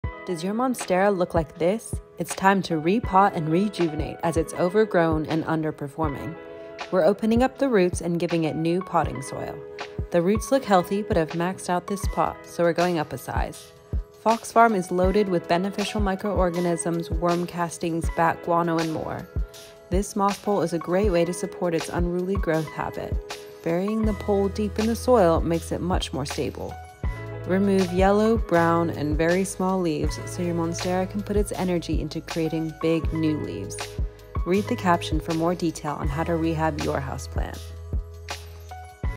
How to Repot a Plant 🪴 Monstera Edition
How to repot a plant, Monstera deliciosa edition 🪴👇

Diagnosis 🩺: This once glorious monstera plant has been delegated to a forgotten corner and its neglect is starting to show. No new growth and lack-luster leaves are the tell take signs of being ready for a repot.

New Soil, new home 🏡: This monstera has been in the same soil for years and has depleted all of its nutrients. A potting soil rich with organic matter and good drainage is ideal for an aroid plant. Going up one pot size means you give a little bit of room for growth but there isn’t too much soil holding unwanted moisture and causing root rot.

Aerial roots - to leave or not to leave? 🌿Aerial roots are neither good nor bad for the plant - in nature they help to climb trees and absorb water from the humid atmosphere. In a home they don’t do much so we trimmed them off for aesthetic purposes.

Water 💦: Always give your plants a good soak when you repot. This helps the soil settle and the roots get acclimated to their new home. Wash down the leaves while you are at it to remove dust and grime that inhibits photosynthesis.

Location, location, location☀️🏡 : This guy is worthy of a prime spot now. Place in bright, indirect light where it can grow & flourish.

Any repotting questions? Ask us in the comments!